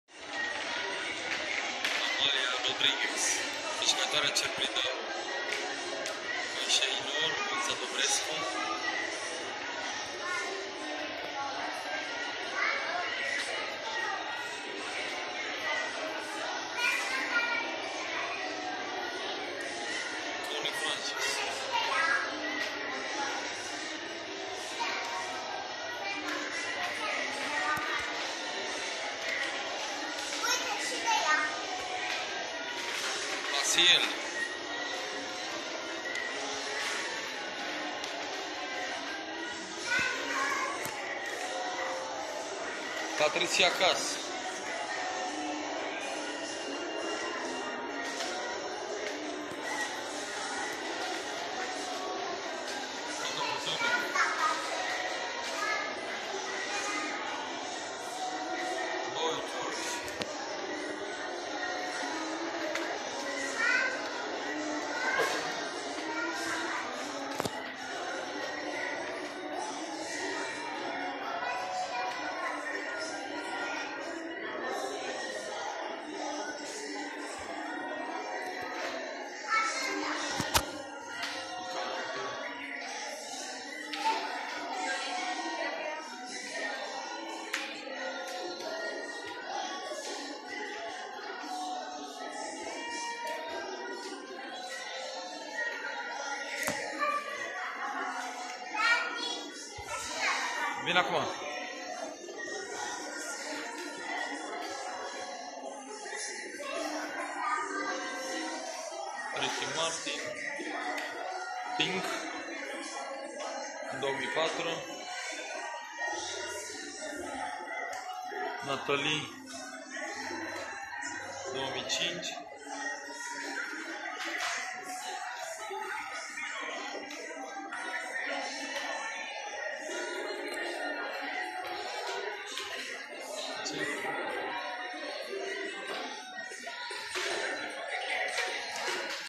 Cerbul de Aur 2018 Brasov
Prezentarea tuturor castigatorilor de la Cerbul de Aur